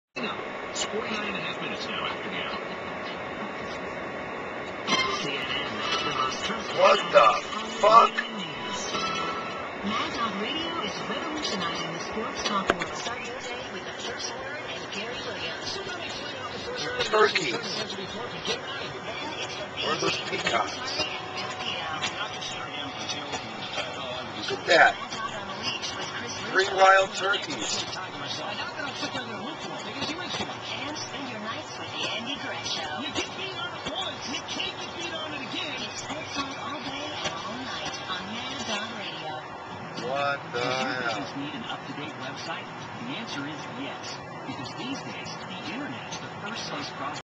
Jive Turkey
I'm drivin' down the road and POW! outta no where these beasts decide to J-Walk...Pretty rare in a major city...LOL